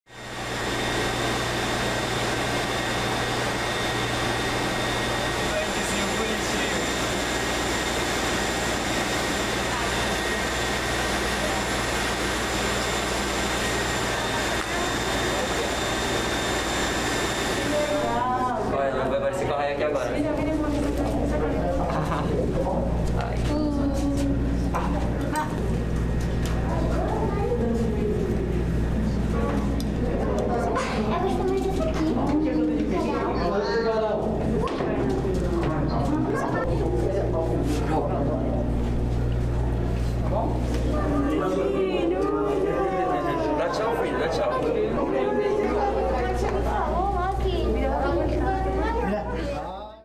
Papa Noel nada con los tiburones en Río de Janeiro
Junto a sus gnomos, se zambullirá en las aguas del acuario Rio Marine, para deleite de grandes y pequeños, hasta el mismo 25 de diciembre durante la hora del almuerzo de los escualos. Volmer Salvador comenta que intentan transmitir un poco de alegría en estos momentos tan difíciles.